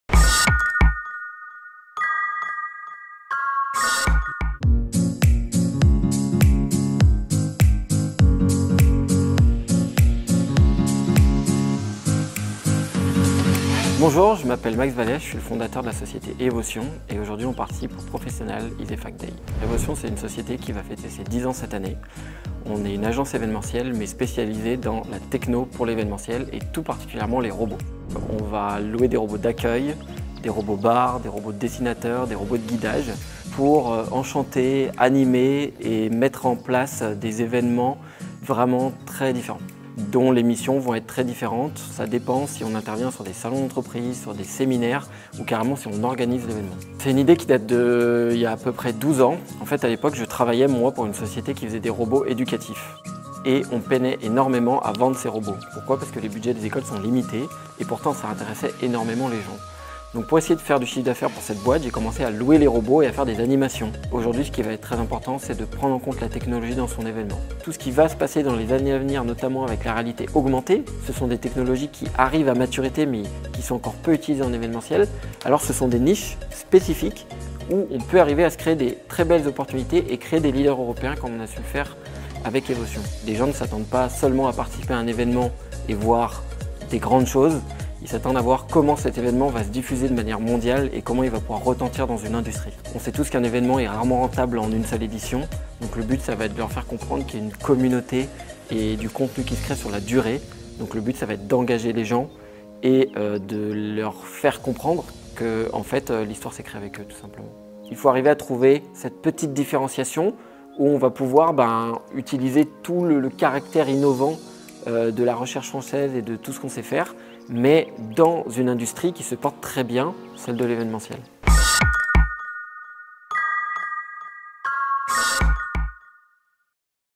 Les robots : la nouvelle tendance événementielle - rencontre avec Max Vallet, fondateur
Tendance importée d’Asie, les robots sont de plus en plus présents au cœur des événements en France ! Qu’ils accueillent les visiteurs, préparent des cocktails en direct, fassent le show, les robots intelligents apportent une touche de dynamisme aux événements.

Ils sont également de véritables stars de la communication ! Leur présence génère une forte visibilité des événements sur les médias sociaux. En plus de générer de l’animation, ils sont donc un moyen de communication très efficace pour les organisateurs.

Les étudiants de l’ISEFAC ont pu échanger sur cette thématique d’avenir avec Max Vallet, fondateur de la société Evotion, dans le cadre des Professional ISEFAC Days du campus de Lyon.